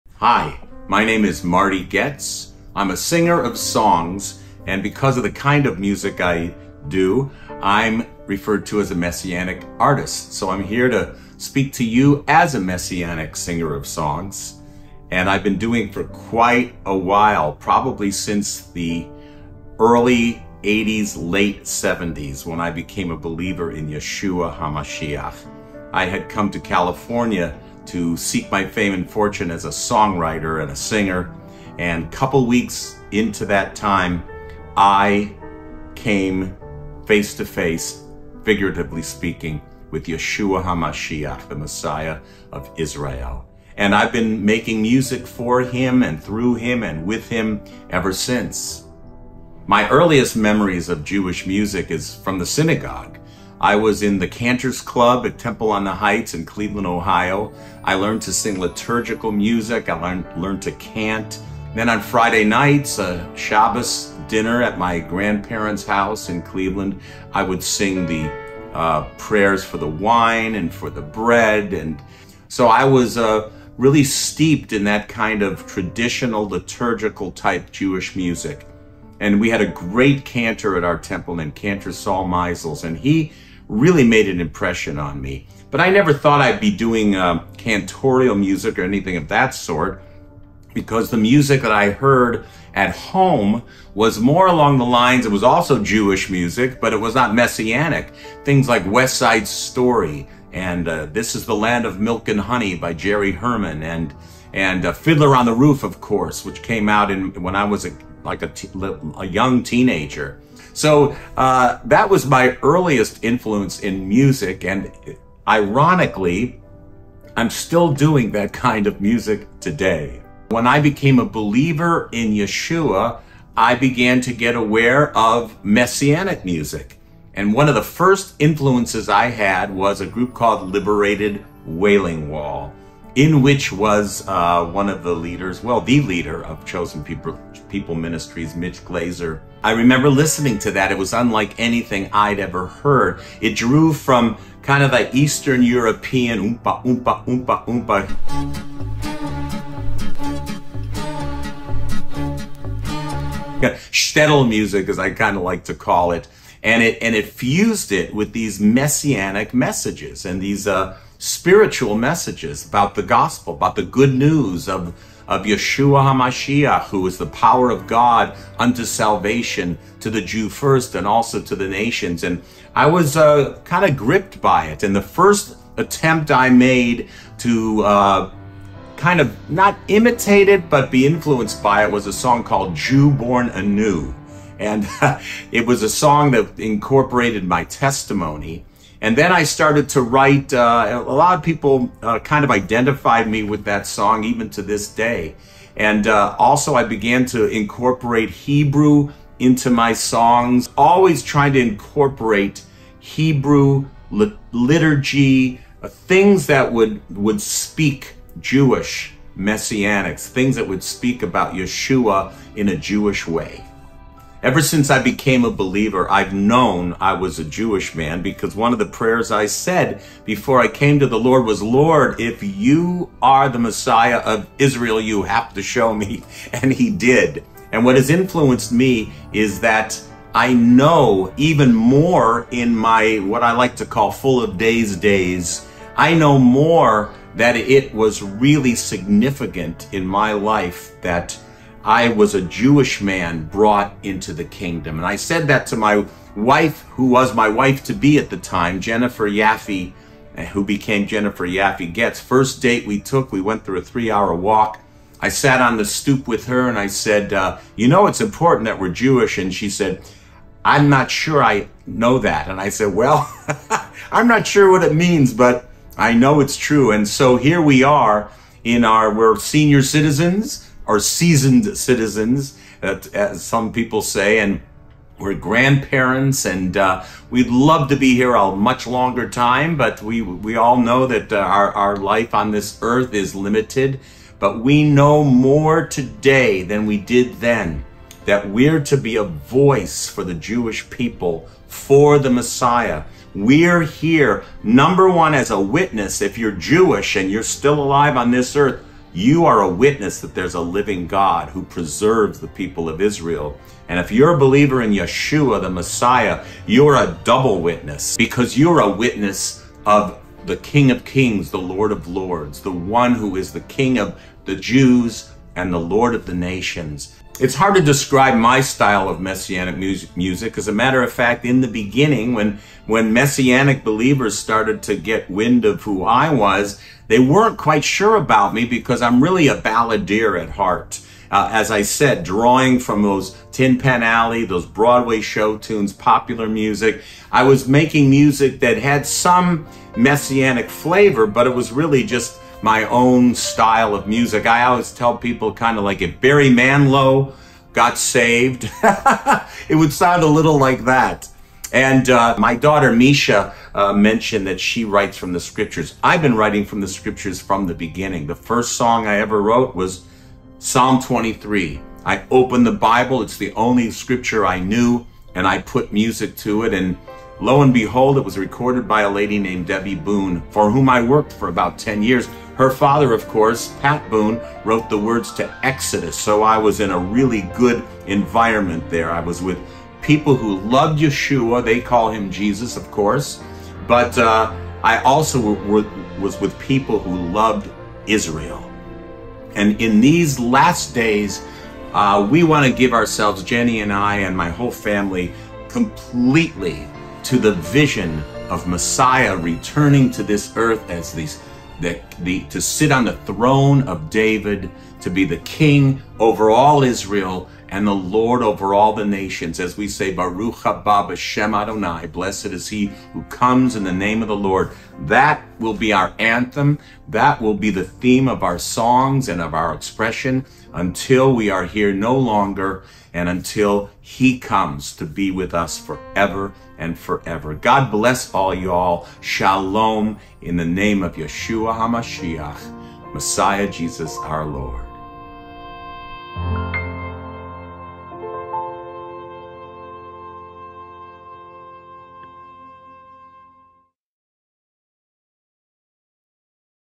Marty Goetz on Writing Messianic Music
Renowned Messianic artist Marty Goetz sat down to chat about his journey as a secular musician entering the world of Messianic worship.